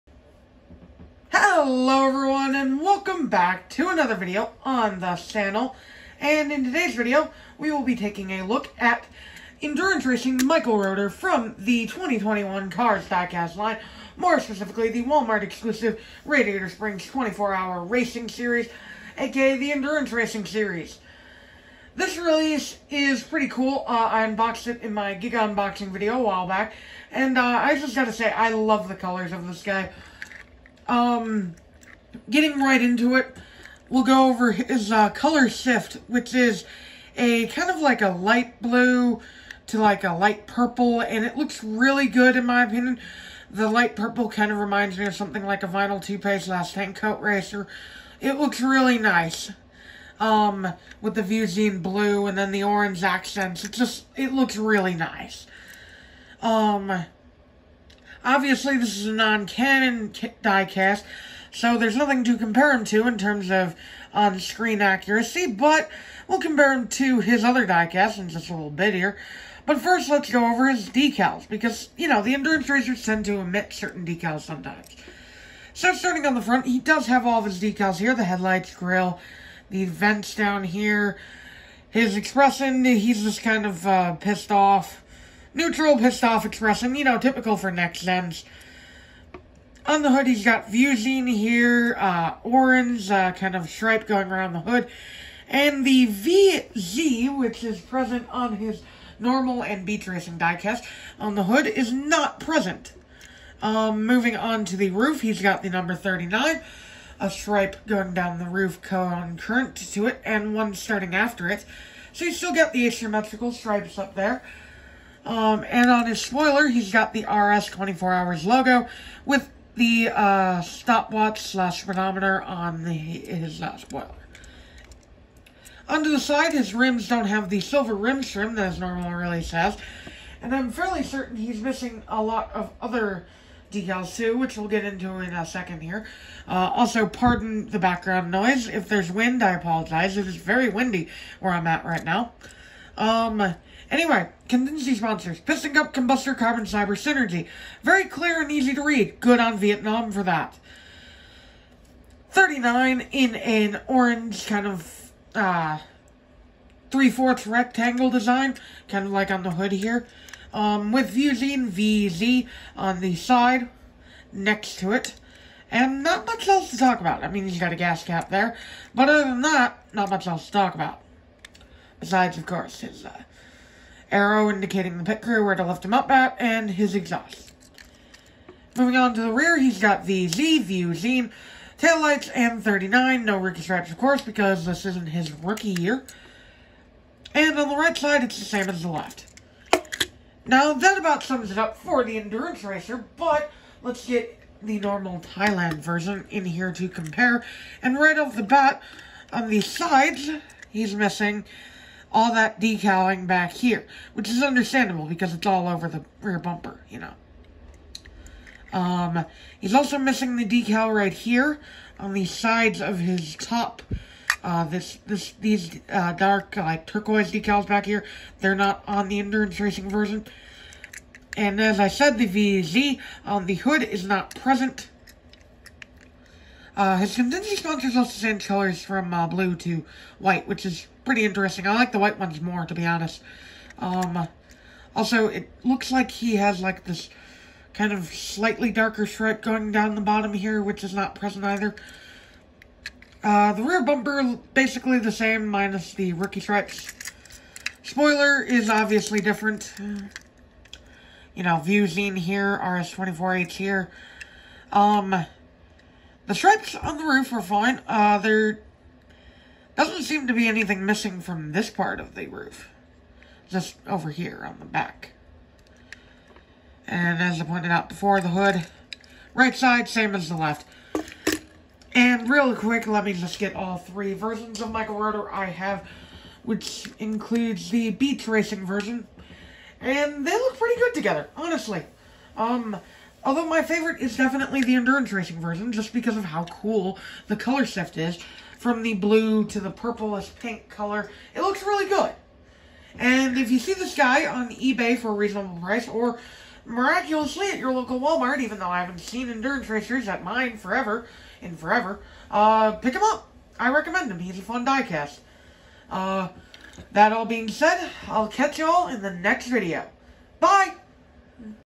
Disney Pixar Cars Quick Review: Endurance Racer Michael Rotor (Next Gen View Zeen #39)
Been a hot minute since we took a look at an Endurance Racer! Today we’ll be taking a look at Endurance Racer Michael Rotor, who is in my opinion the best looking Endurance Racer Mattel has made. What do y’all think?